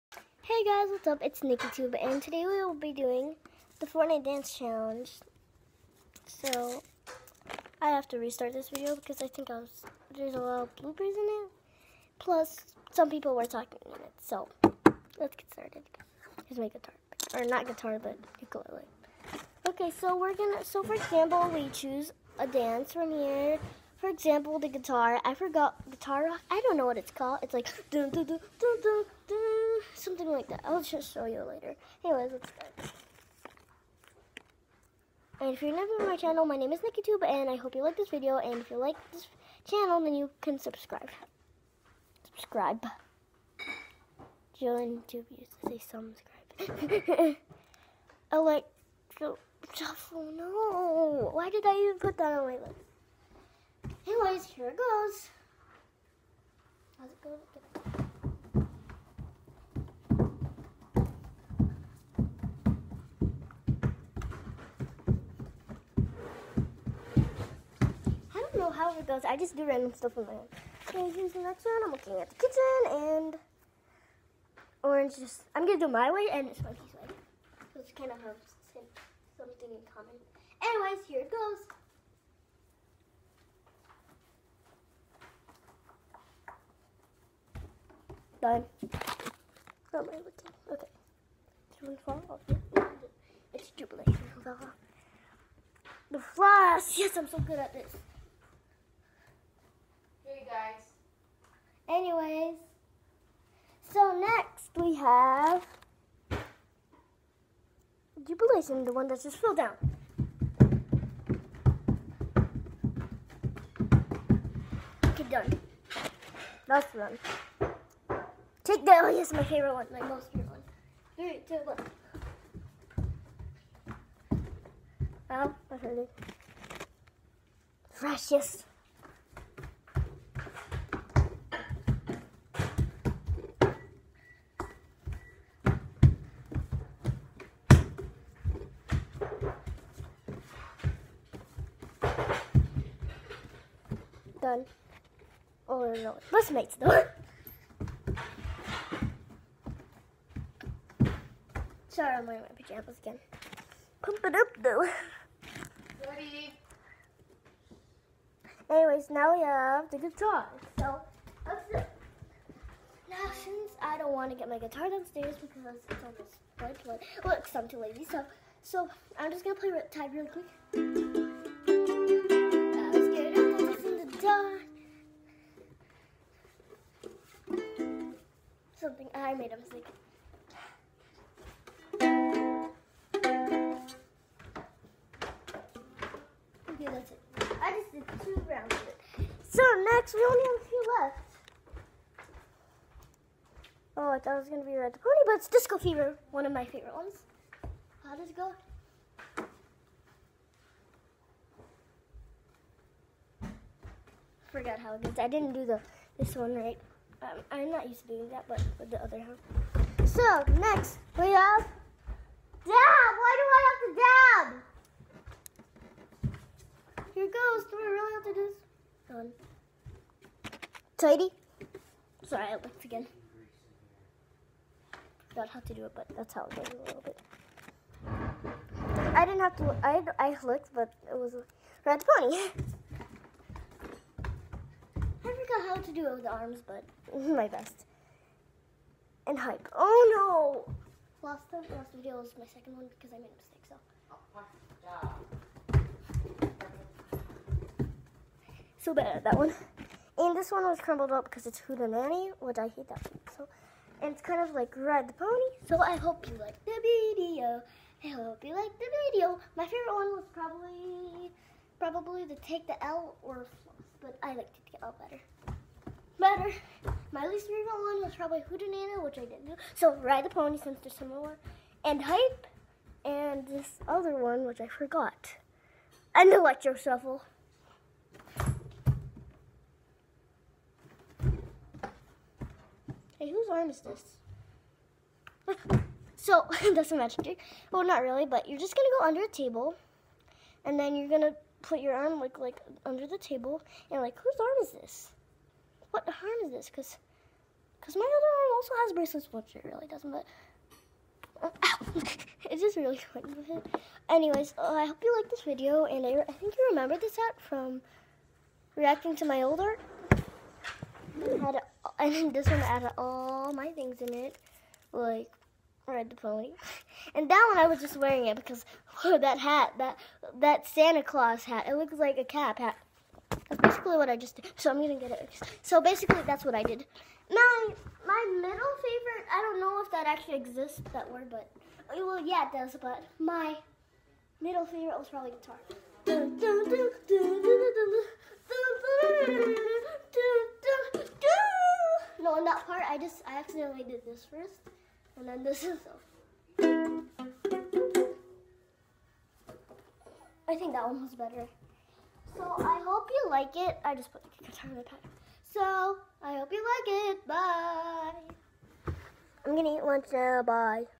FORTNITE DANCES
Hype, Floss, Guitar 🎸 Rock. Ride that pony, Disco Fever, Best mates. Take The LLL haha im kidding just L. Find out my other dances in the video. 🤪😜🦄💁🏻‍♀️💋Nikki from Ntube!